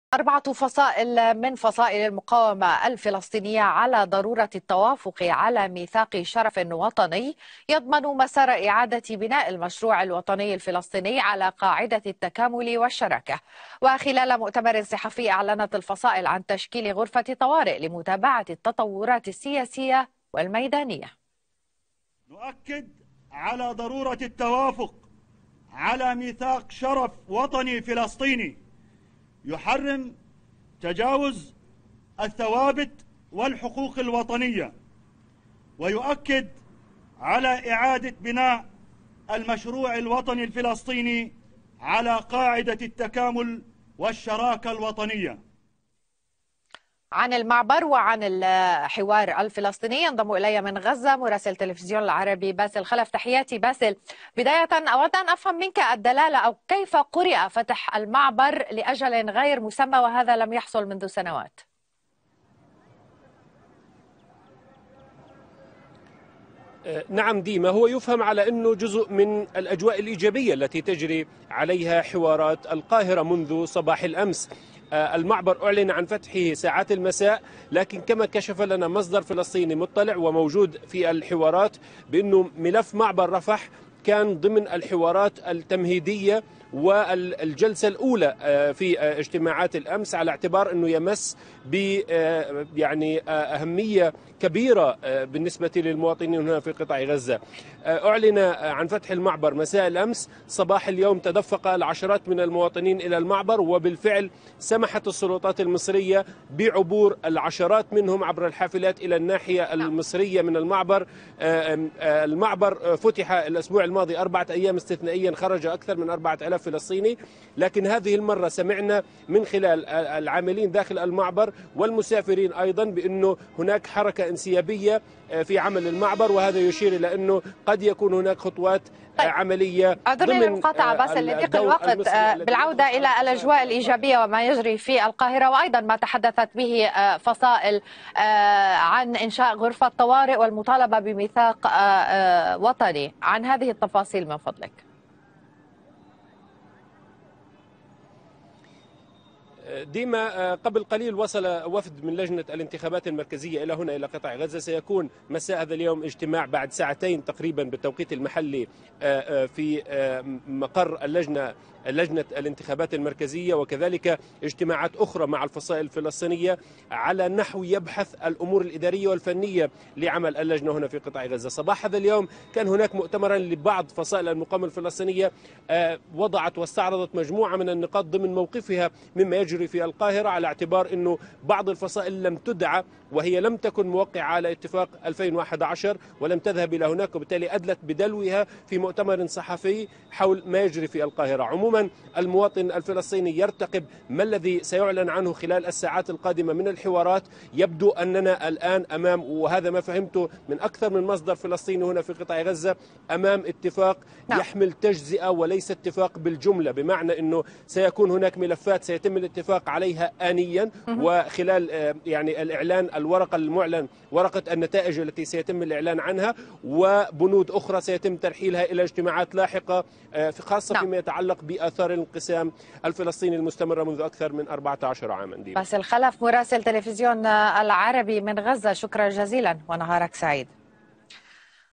فصائل فلسطينية تتفق على ضرورة التوافق على ميثاق شرف شرف وطني | أخبار العربي
عقدت 4 فصائل فلسطينية وهي حركة المجاهدين الفلسطينية ولجان المقاومة في فلسطين وحركة الأحرار الفلسطينية وجبهة النضال الوطني لقاء وطنيا للتباحث في القضايا المطروحة على الساحة أكدت من خلاله ضروره إقرار ميثاق شرف يضمن تحقيق الوحدة الوطنية الشاملة.